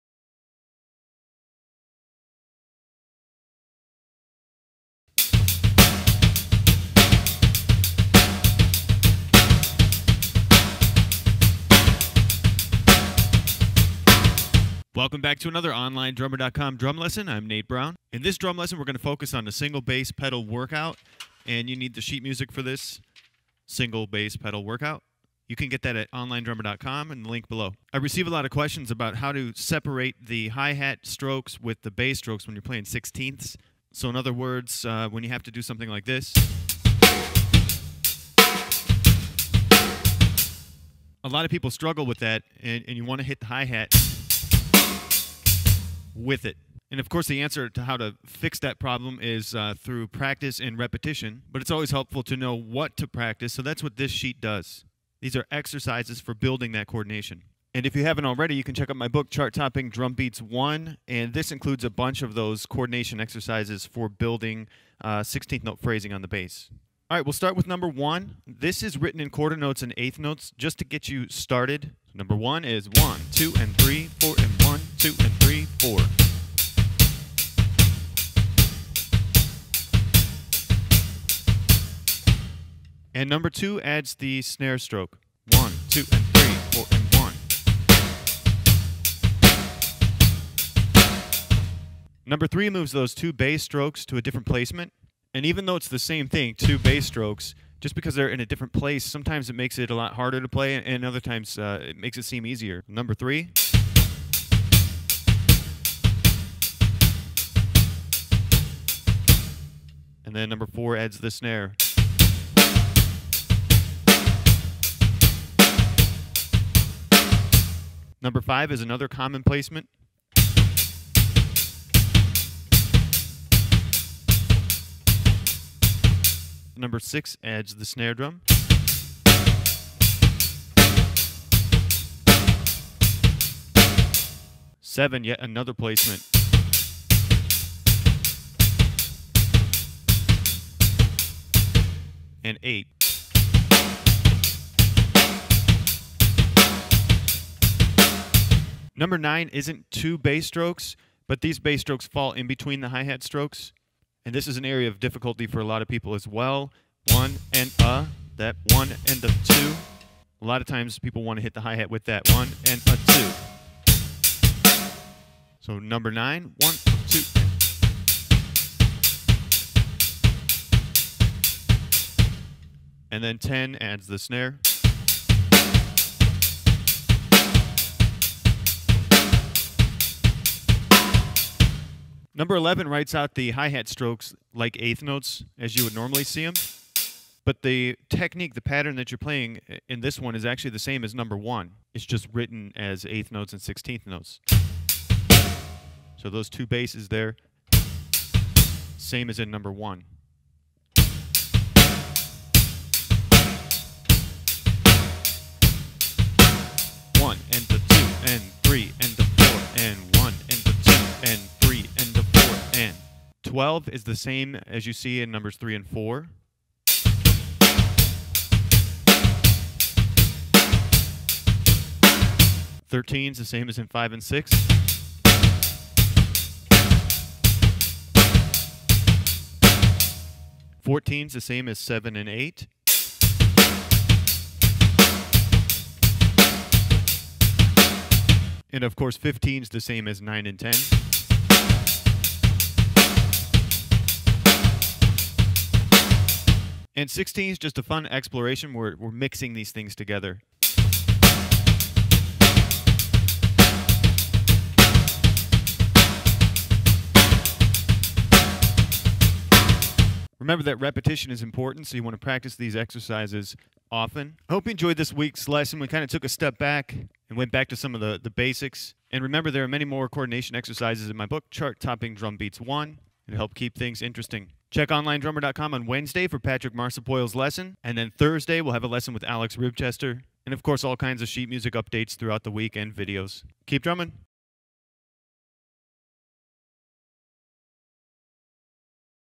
Single Bass Pedal Exercises - Drum Lesson
I receive a lot of questions asking for ways to help unlock the hi-hat hand from the bass drum foot. The answer is – repetition. The more you play these types of patterns, the easier it will be to keep consistent hi-hat strokes while playing 16th notes on the bass drum.